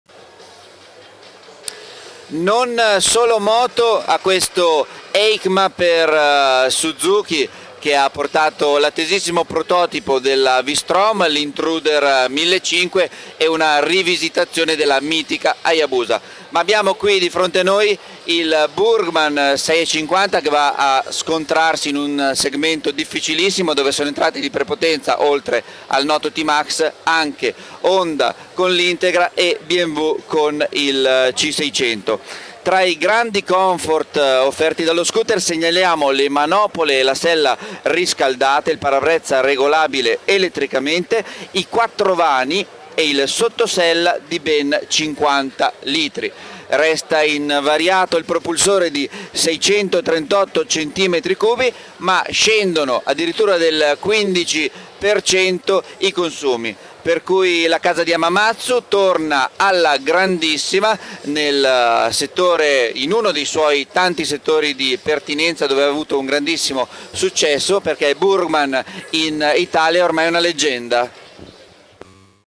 Suzuki Burgman 650 e le novità di Eicma 2012
V-Strom 1000, Intruder 1500 e Burgman 650: sono queste le novità di Suzuki che vi racconta Federico Aliverti in diretta da Eicma